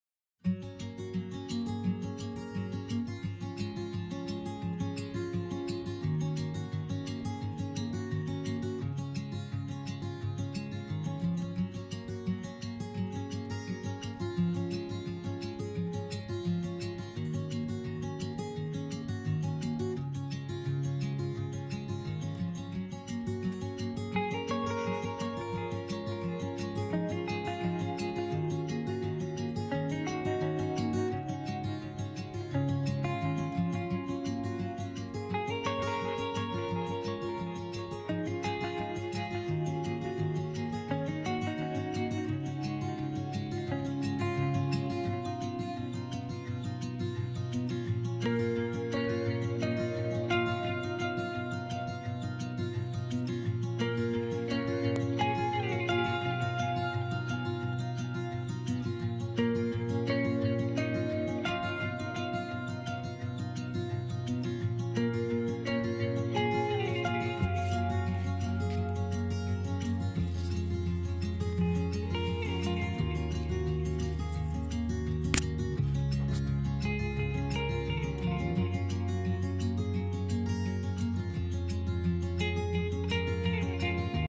🥒🥒🥒 CUCUMBER / PIPINO QUICK GERMINATION
Hi Everyone
A short video showing of how I germinated Cucumber / Pipino